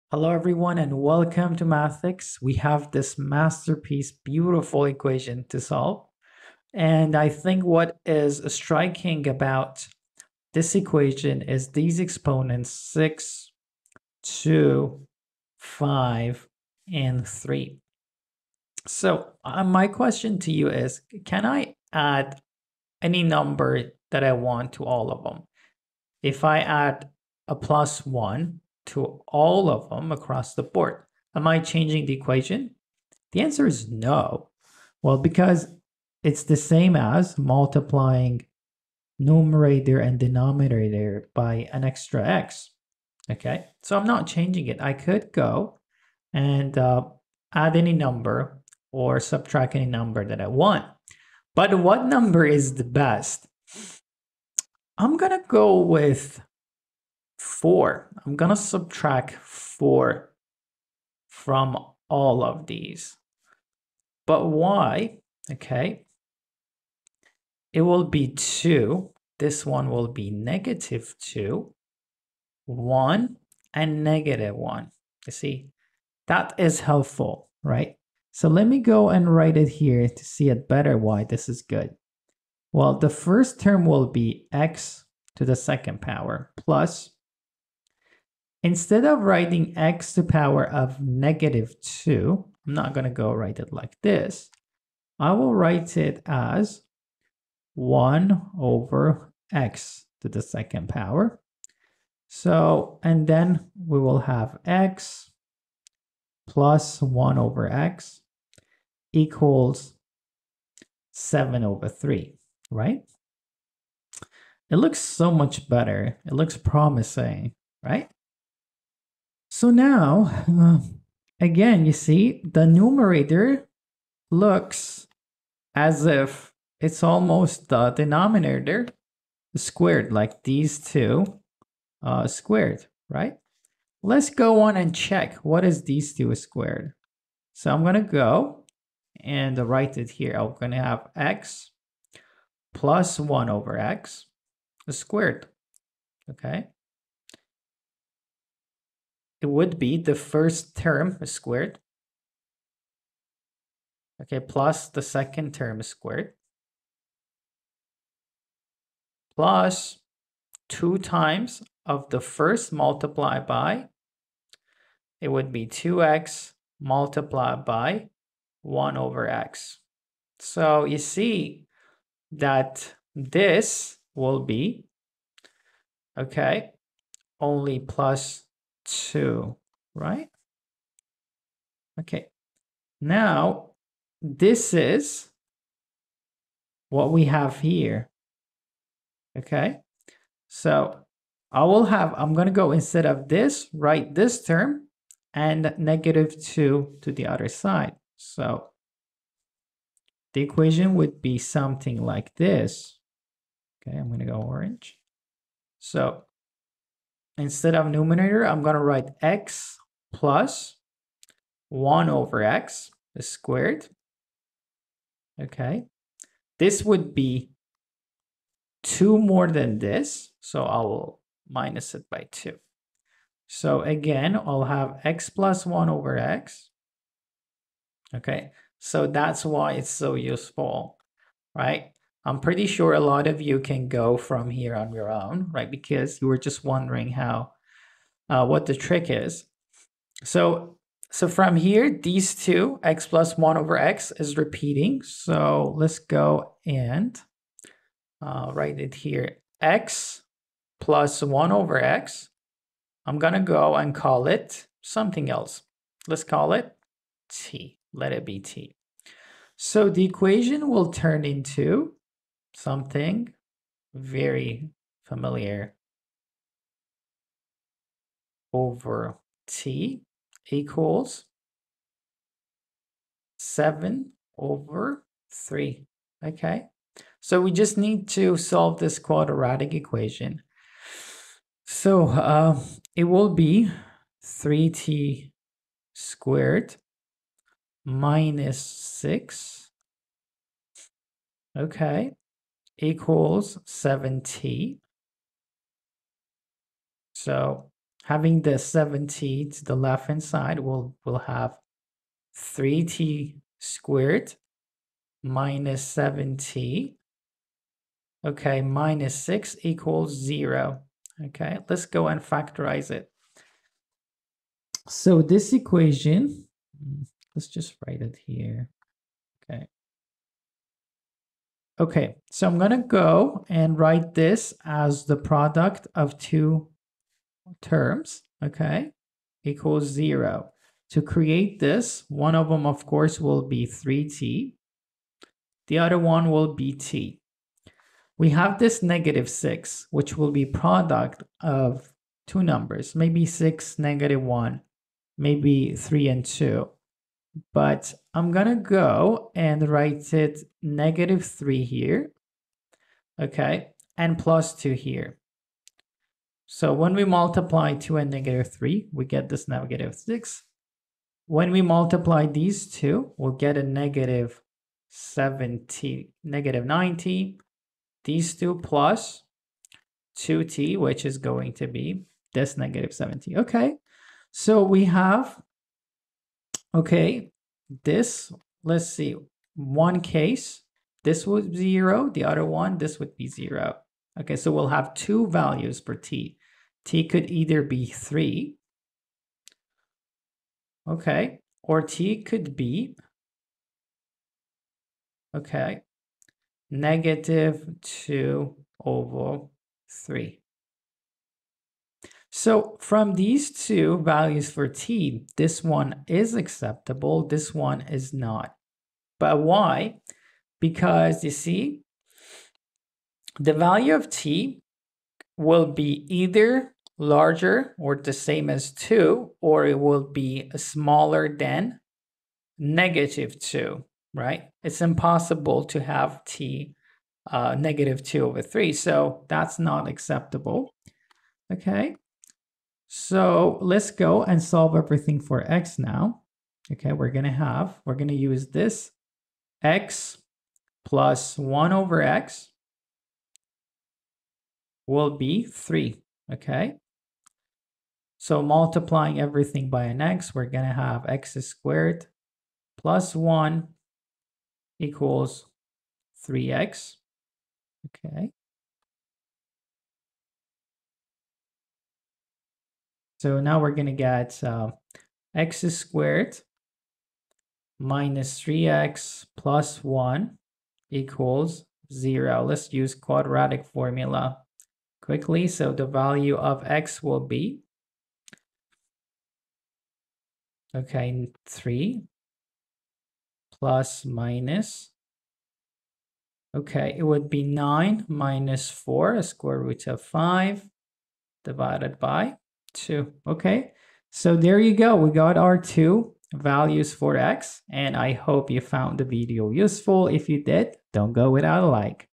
A Super Tricky Equation to Solve | Olympiad Training
4 minutes is enough to solve the equation if you know what you're doing.

(X^6 + X^2) / (X^5 + X^3) = 7/3

This is not an actual IMO question. I designed this problem to prepare for International Math competition.